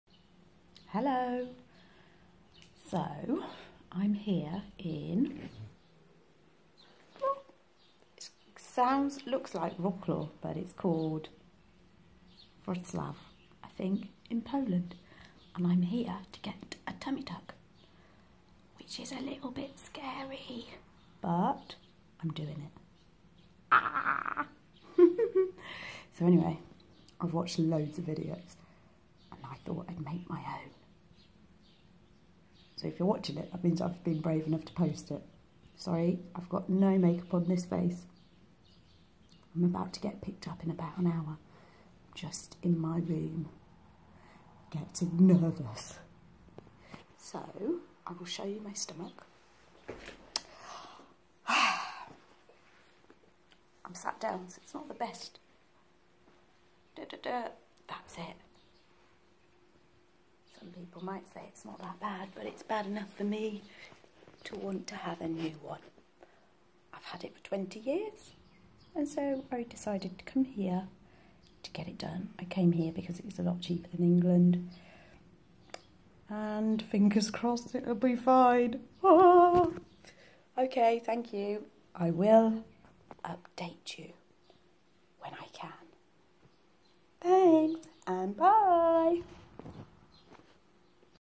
The day of tummy tuck poland no.1
My short video clip before tummy tuck surgery